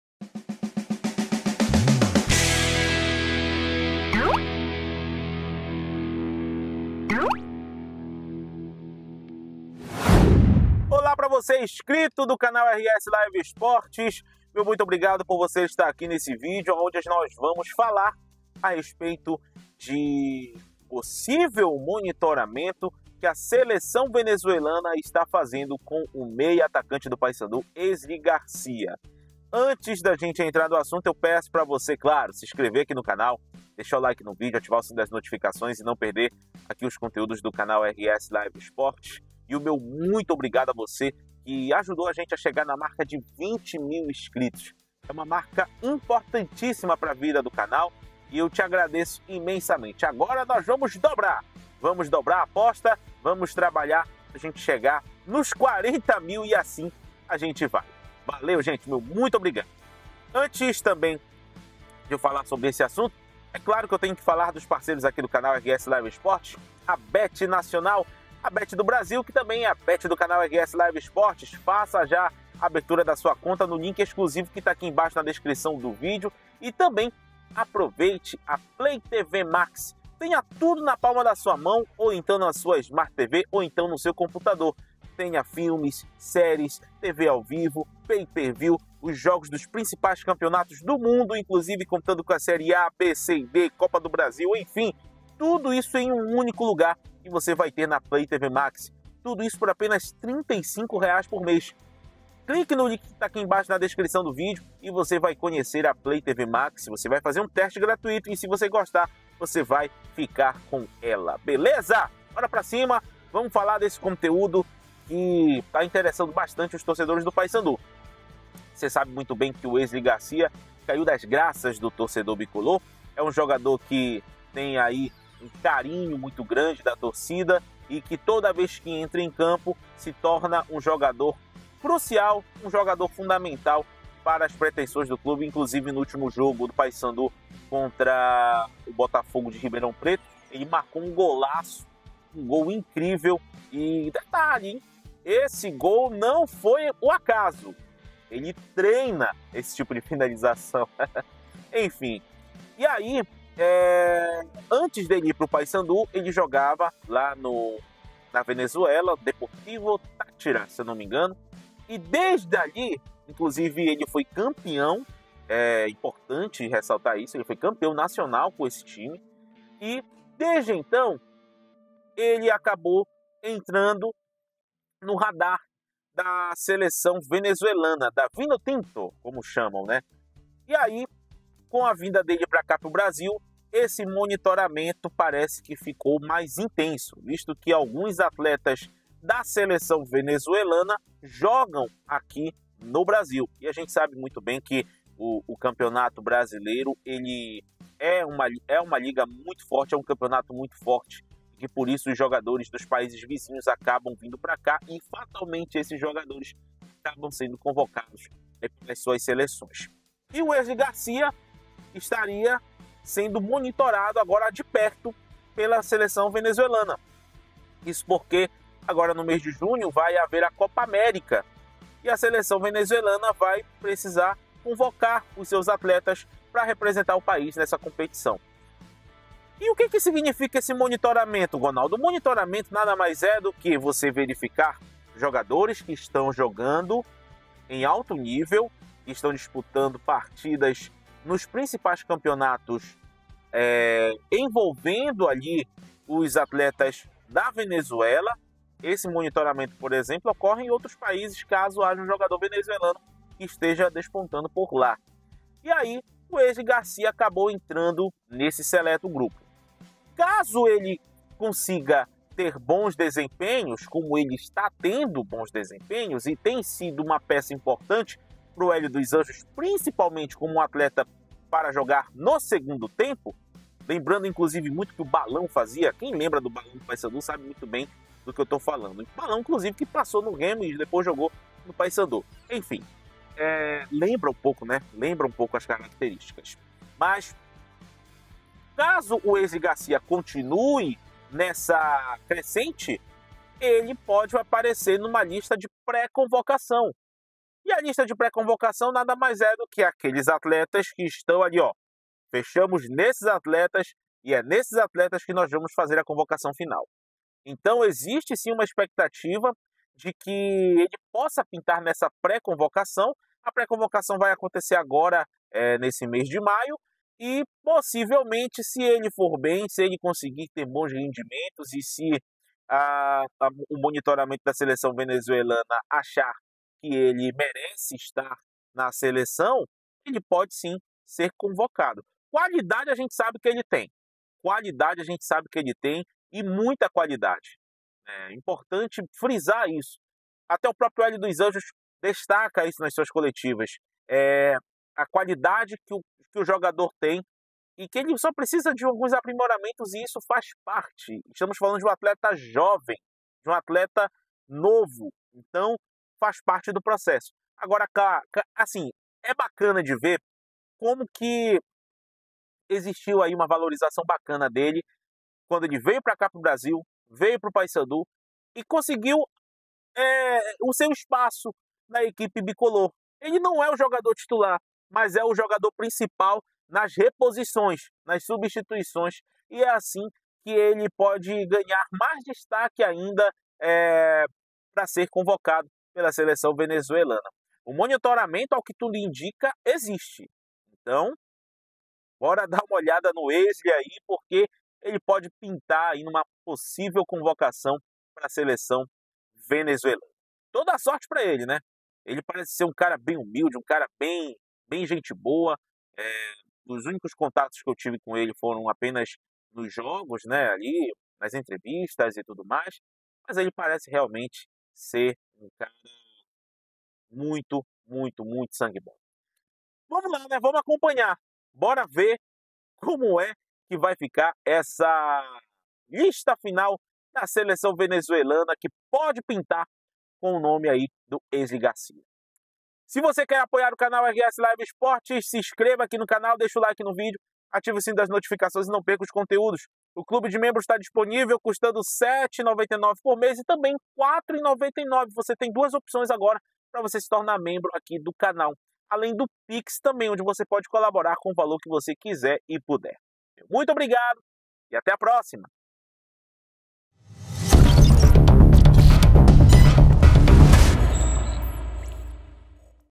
ESLI GARCIA, DO PAYSANDU, ESTÁ SENDO MONITORADO PELA SELEÇÃO VENEZUELANA? CONFIRA!
Play TV Max - Tenha canais ao vivo, filmes e séries em um único lugar